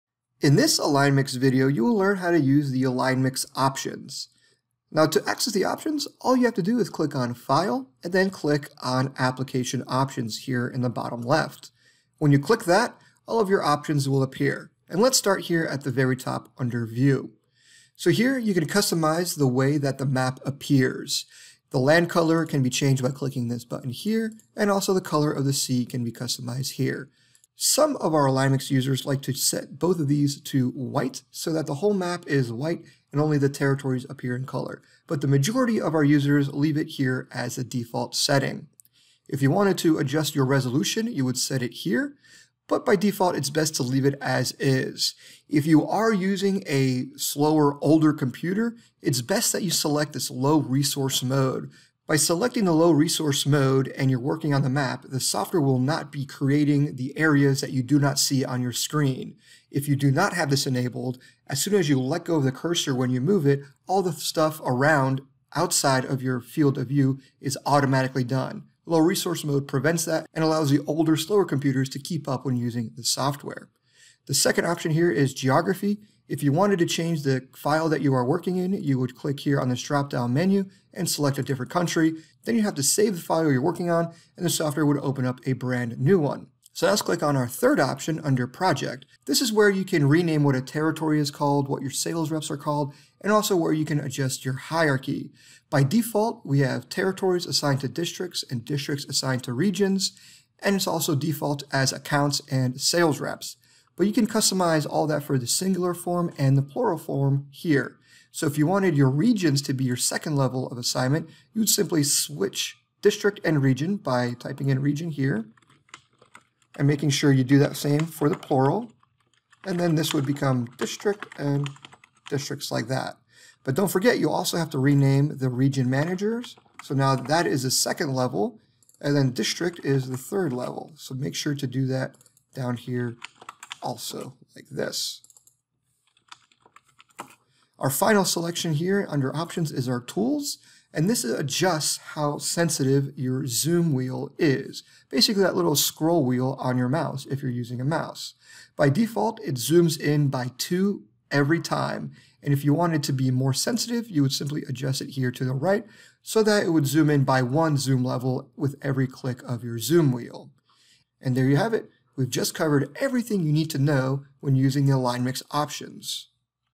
AlignMix Options
The Alignmix options allows you to customize the appearance and terms used in the AlignMix mapping software.